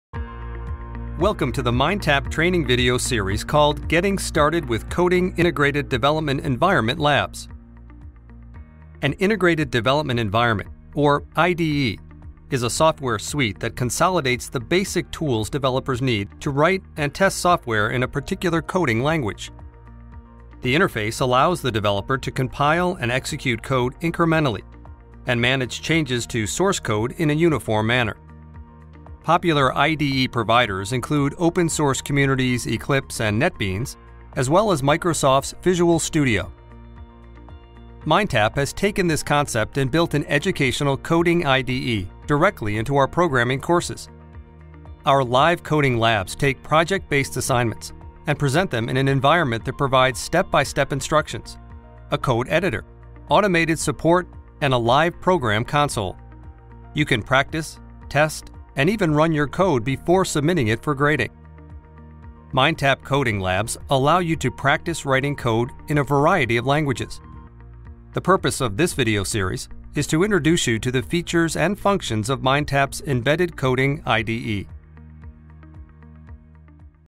Getting Started with Coding IDE Labs
This video provides insight about IDE Labs and testing software.